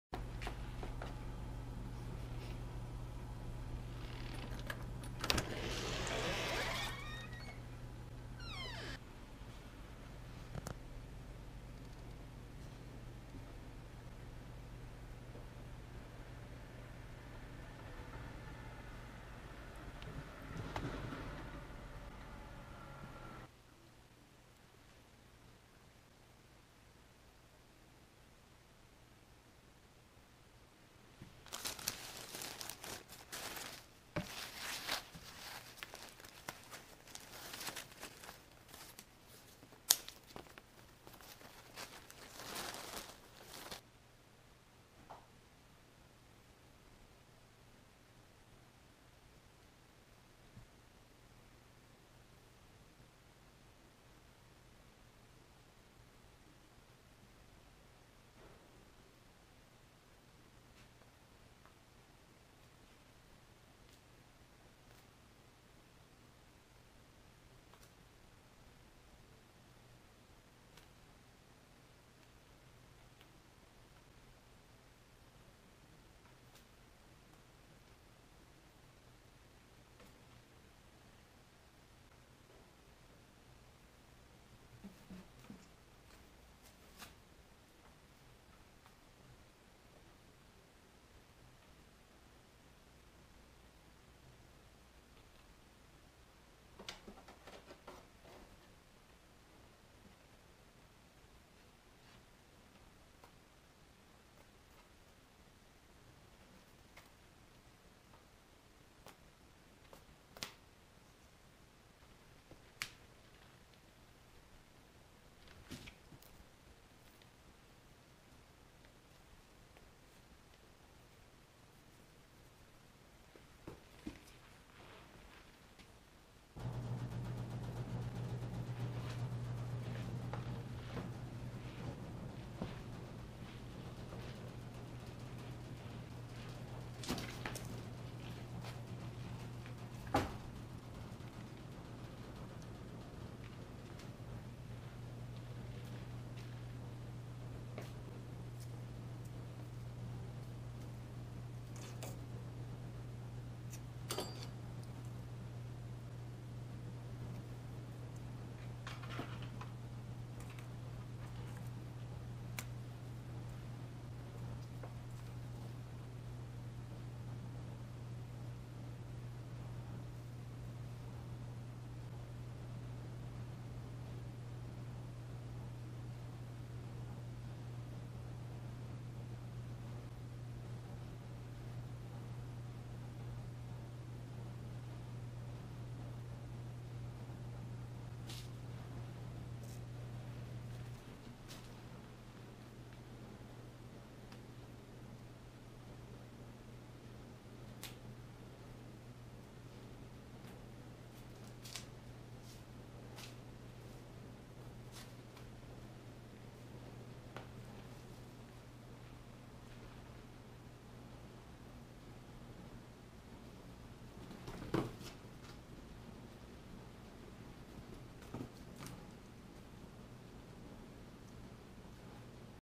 Improved Cat Toy (Caulk Saver = FUN)
You don't have to spend money on gimmicky store-bought toys.
Caulk Saver is Joe Approved.